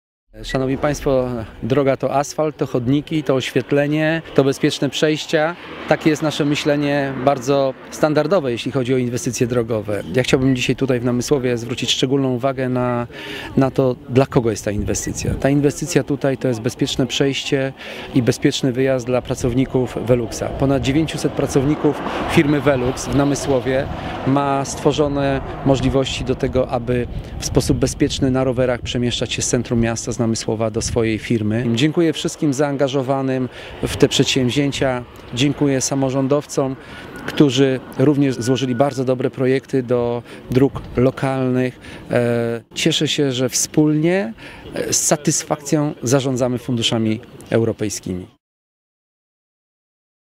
Bezpieczny dojazd do Fabryki Velux w Namysłowie
„Ta inwestycja, to bezpieczny dojazd do Fabryki Velux w Namysłowie. Wielu pracowników tego zakładu korzysta z rowerów. Teraz mogą bezpiecznie się przemieszczać” - powiedział Marszałek Województwa Opolskiego Andrzej Buła.

Wartość całkowita projektu to 2,95 mln zł. Wkład Gminy Namysłów to 370 000 zł przeznaczonych na prace budowlane i 80 000 zł na dokumentację. Droga przy której nasadzono dziś drzewa, została kompleksowo przebudowana. Przygotowano zjazdy do strefy ekonomicznej. Według badań drogą każdego dnia przejeżdża około 5100 pojazdów.